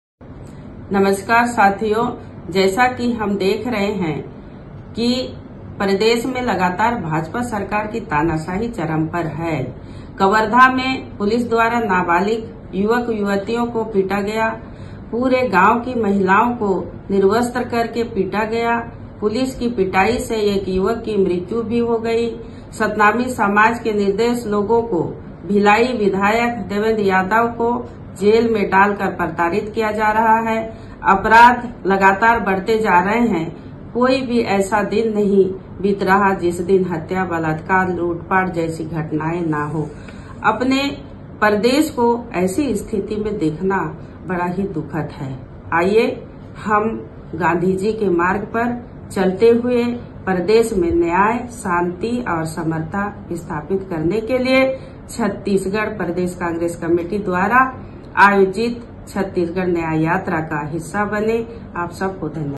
गिरौदपुरी से शुरू होने जा रही, छग न्याय यात्रा' में सम्मिलित होने सांसद फुलों देवी नेताम की अपील
27 सितंबर गिरौदपुरी से शुरू होने जा रही 'छत्तीसगढ़ न्याय यात्रा' में सम्मिलित होने राज्यसभा सांसद श्रीमति @NetamPhulodevi जी की अपील